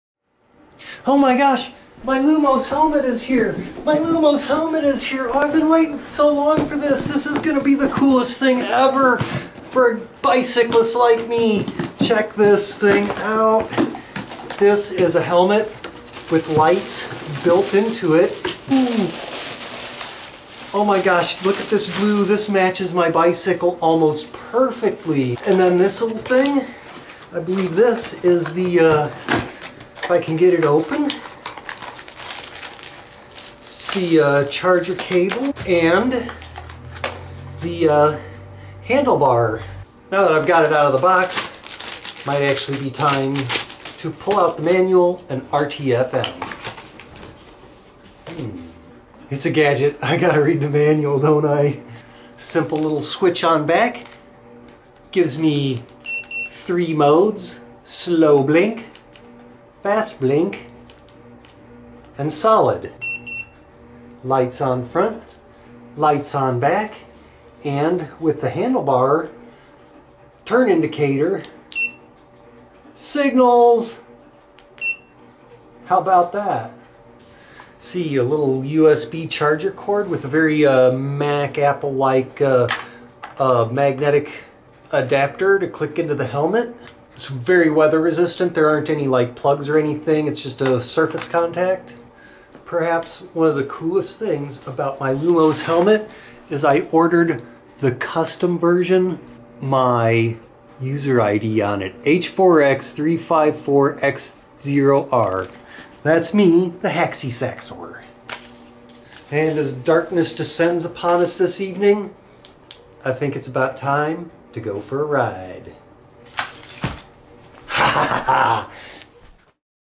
Lumos Helmet Unboxing
Merry Christmas to Me - My new Lumos Helmet is Here! Watch me unbox the COOLEST FREAKIG BIKE HELMET ON THE PLANET!